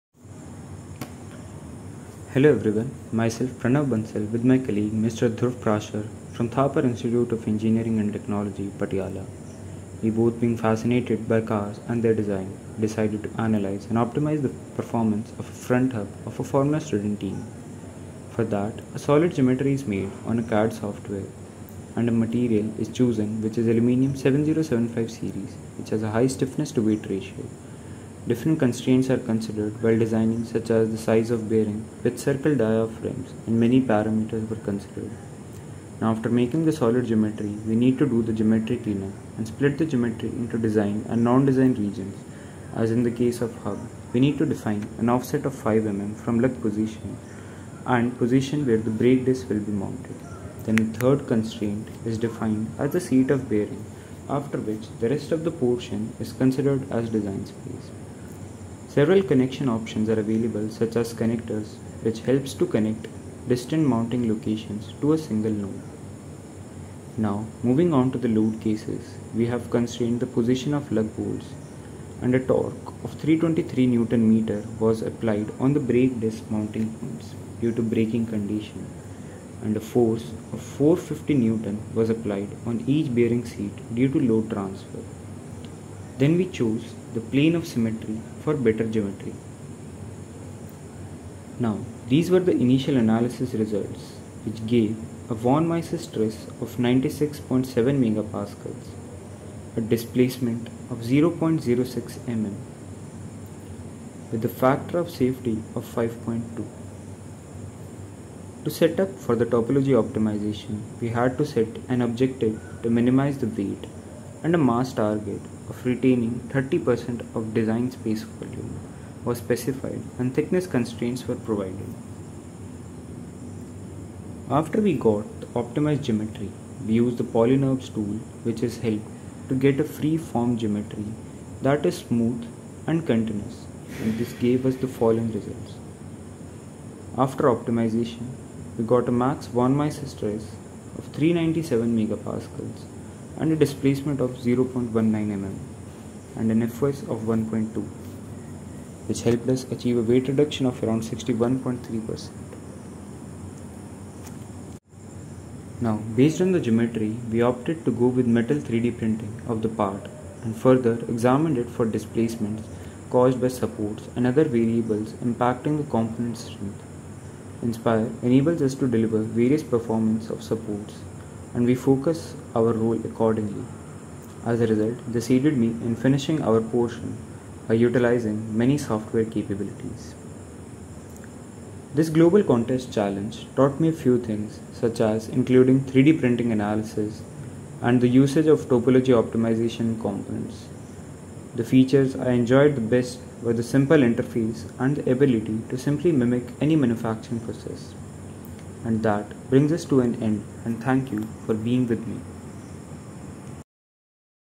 Pranav Bansal and Dhruv Prasher - APAC Winner for August 23 - Altair Global Student Contest
Congratulations to Pranav Bansal and Dhruv Prasher, August’s APAC champion of the Altair Global Student Contest! 🌟 Your outstanding skills and innovative ideas have set you apart from an exceptional pool of talented students.
Pranav Bansal and Dhruv Prasher are 3rd-year undergraduate Mechanical Engineering students from Thapar Institute of Engineering and Technology in India with a keen interest in Computer Aided Engineering (CAE) practices. They chose to show us the great work they did on a front hub from their team’s Formula Race Car. The two of them set up some detailed design spaces and not only optimized their hub but then went the next step to use Inspire’s Polynurbs tool to create a beautiful and functional design. Their optimized design reduced the hub’s mass by an incredible 61.3% while maintaining a respectable factor of safety.  There is no doubt that their car is going to be a force to reckon with in the 2024 FSAE competitions, way to go!!